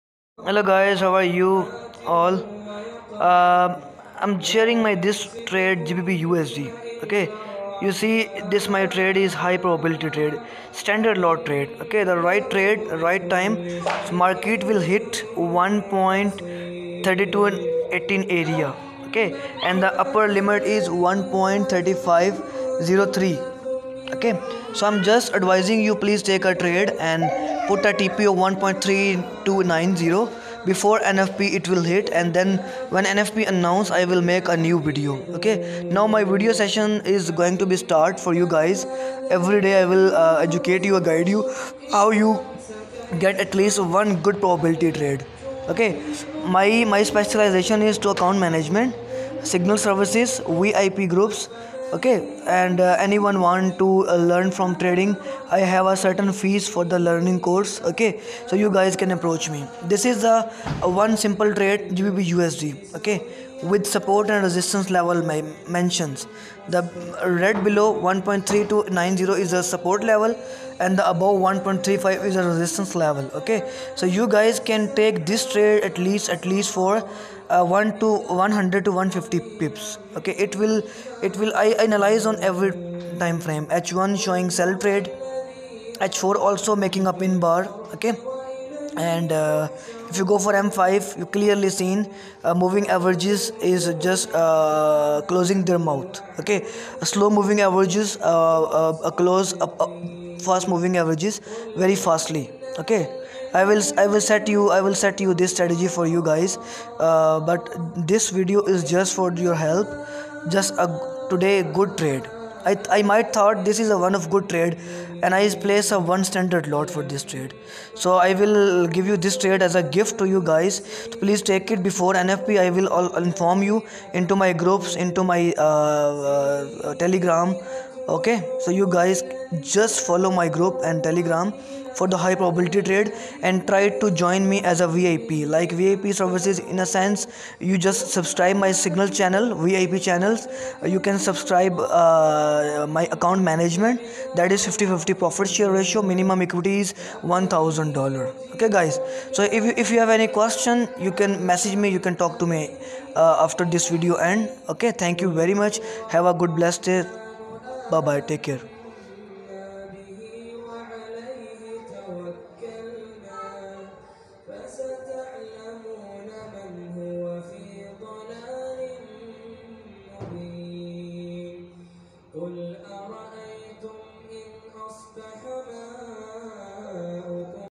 GBPUSD high probability trade before nfp
gbpusd reject the level from 1 .3500 it is now going towards sel target 1.3290 and it will give u good pips ...